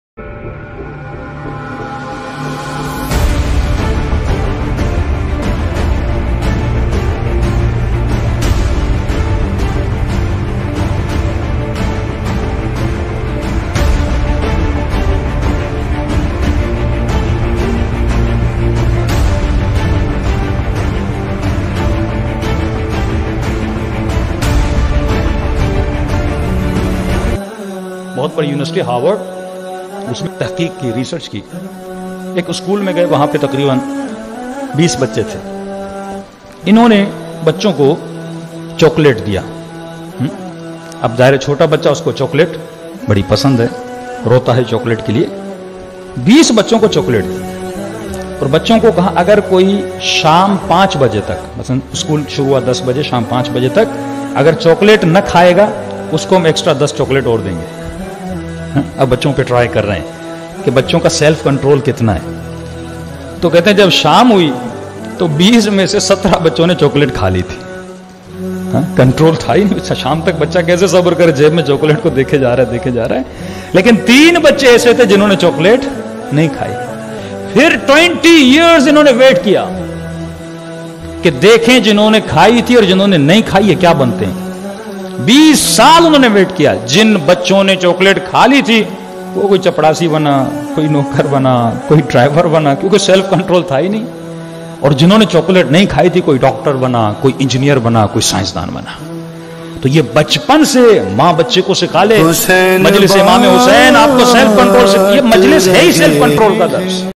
SELF CONTROL || Research by Harvard University || Molana Syed Arif Hussain kazmi
Best research is done by Harvard University of his children  about to know the power off the self control.
this research story is explained by Molana Syed Arif Hussain kazmi .
My request is to listen and watch this video very very carefully .
Also Share this marvelous story to your friends and also to family.